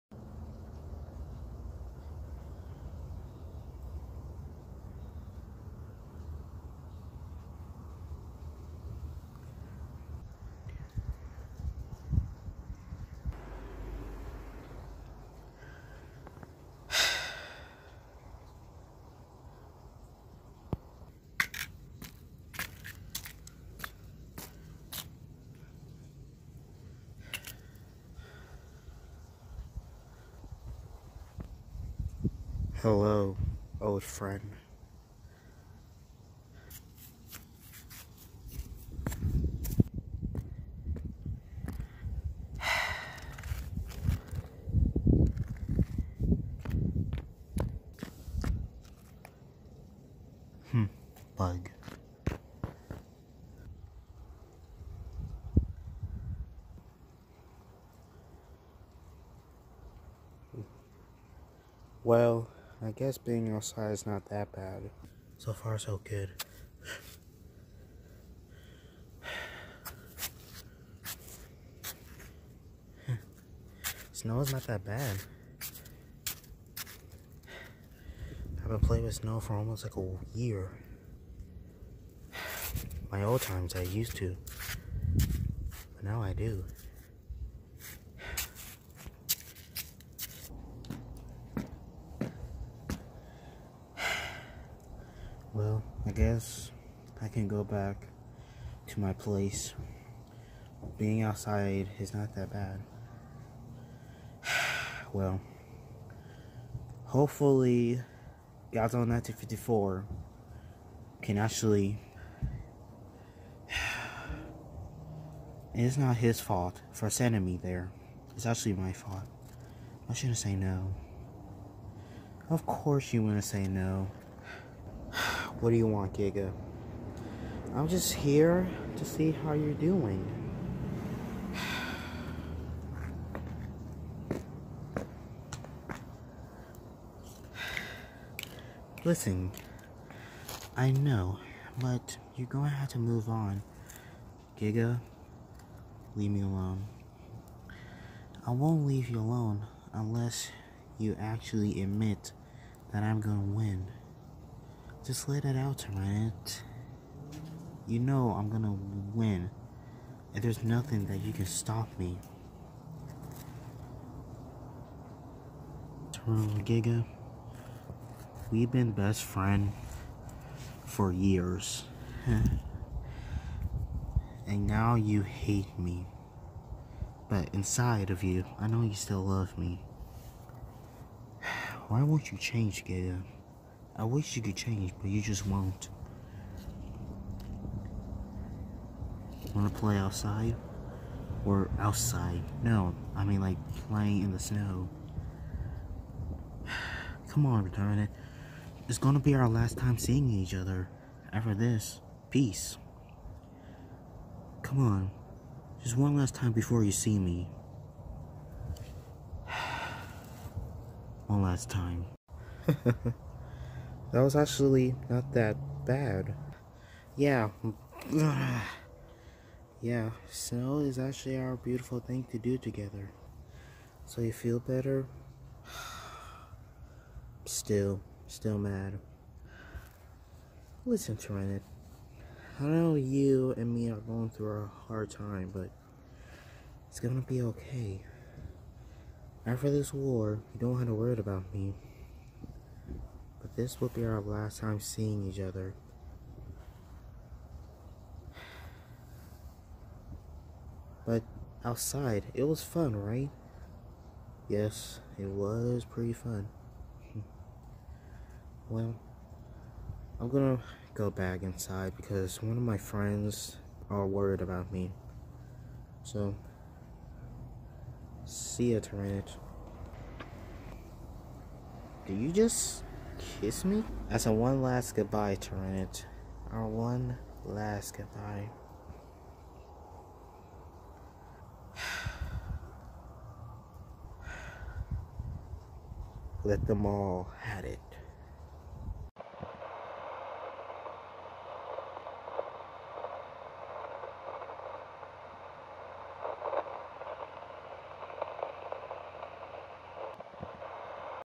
Godzilla and rexy season 12 episode 17 friends and foes
Tyrant is alive but playing in the snow after he was hurt and now he had to rest down. But his worst/friend giga came what will happen next???